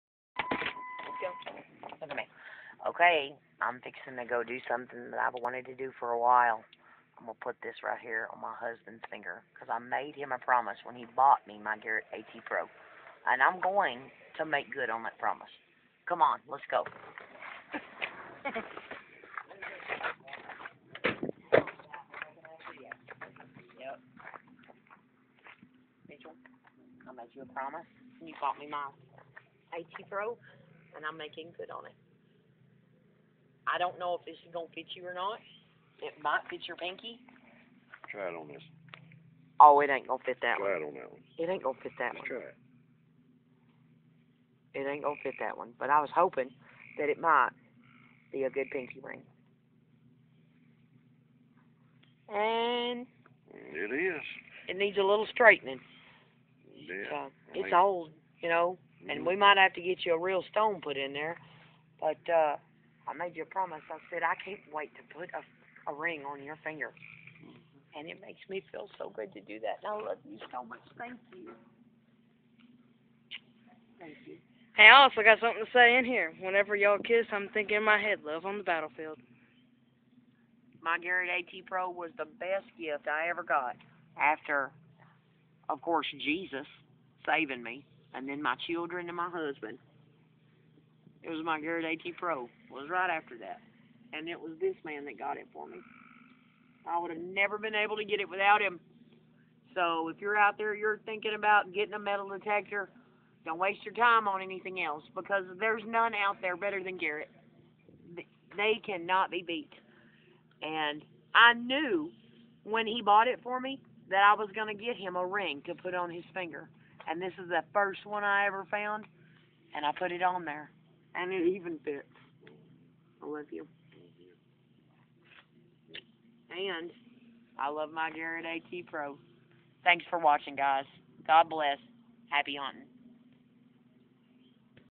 AT PRO ring find makes me a promise keeper! Metal detecting with the Garrett AT PRO
I told him when he got me my AT PRO that I COULDN'T WAIT to put a ring on his finger! In fact, I promised I would and today I made good on that promise! ILY Mitchell Smith GOLD RING is next I hope!